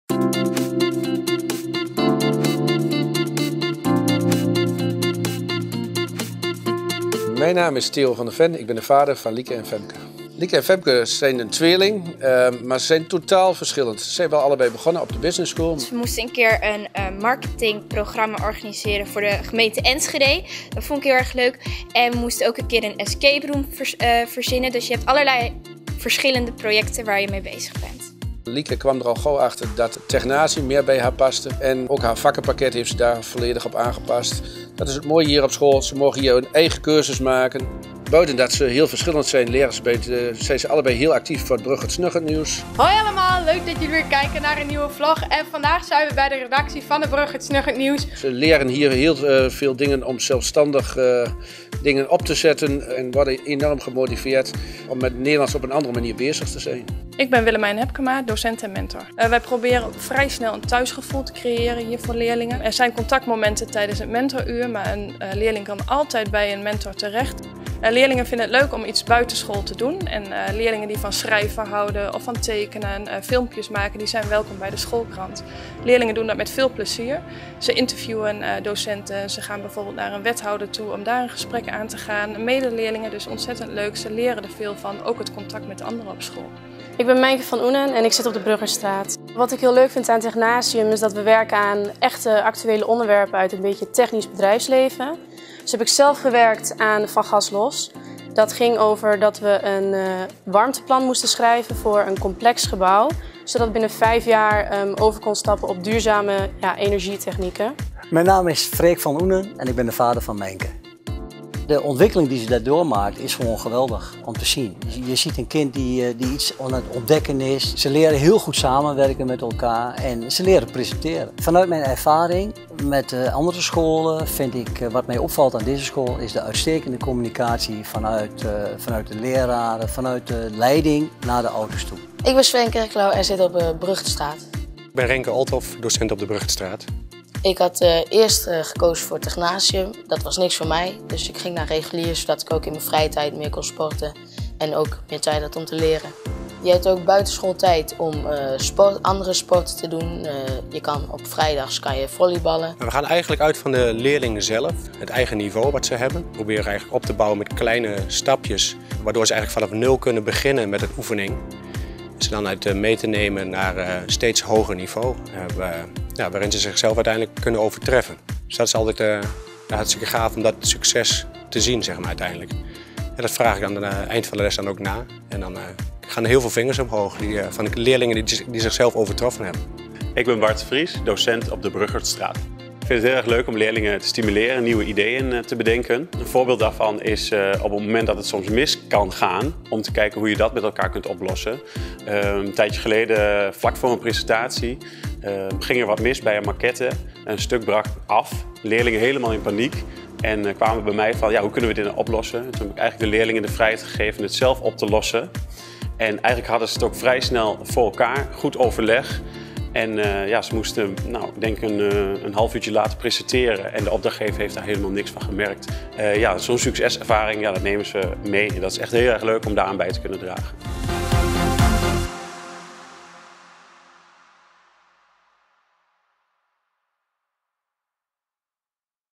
Bonhoeffer College Bruggertstraat
Bonhoeffer College Bruggertstraat (voor ouders)

Waar gaat uw kind naar toe?

Wanneer uw zoon of dochter dit schooljaar in groep 8 zit, dan bent u inmiddels - samen met uw kind - aan het nadenken over het vervolgonderwijs. Een spannende tijd voor uw kind. Maar zeker ook voor u. Want kies maar eens precies díe school die het best bij uw kind past. Waar het zich helemaal thuis voelt en uitgedaagd wordt om zichzelf en de wereld te ontdekken.

Op Bonhoeffer College Bruggertstraat gaat dat zeker lukken. Op een eigentijdse manier gaan we op zoek naar wie uw kind is, wat het goed kan en welke dingen hij of zij echt leuk vindt. Samen grenzen verleggen en talenten ontwikkelen.

In deze video kunt u alvast kennis maken met onze school.

Meer weten over het Bonhoeffer College? Kijk dan op